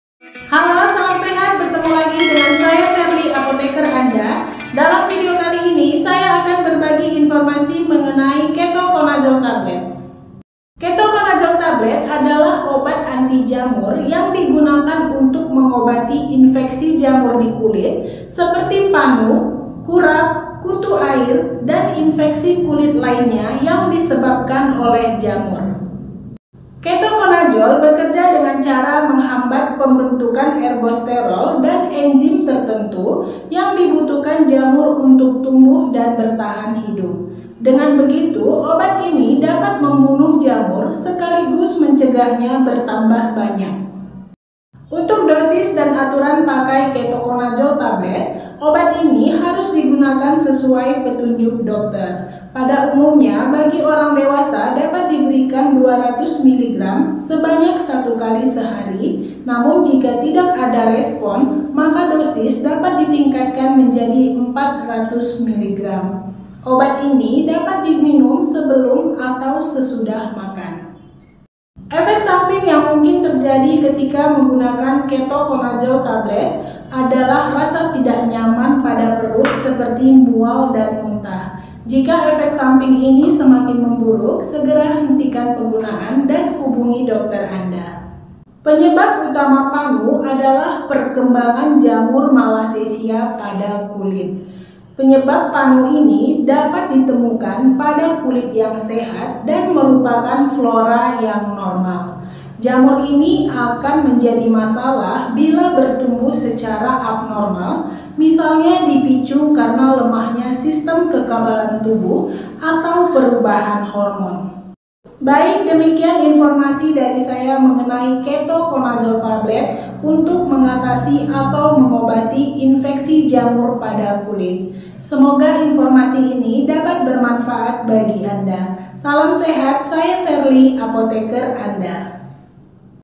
Obat Infeksi Kulit - Panu - Kurap - Kutu Air || Ketoconazole
Ketoconazole
facebook twitter share to email line chat whatsapp
Ketoconazole adalah obat antijamur jenis azole. Obat ini digunakan untuk mengobati infeksi jamur di kulit, seperti panu, kurap, kutu air, kandidiasis, dermatitis seboroik, dan ketombe yang berkaitan dengan jamur.

Ketoconazole bekerja dengan cara menghambat pembentukan ergosterol dan enzim tertentu yang dibutuhkan jamur untuk tumbuh dan bertahan hidup. Dengan begitu, obat ini dapat membunuh jamur sekaligus mencegahnya bertambah banyak.

Ketoconazole juga bisa digunakan untuk mengobati infeksi jamur sistemik, yaitu infeksi jamur yang dapat mengenai beberapa organ dalam tubuh.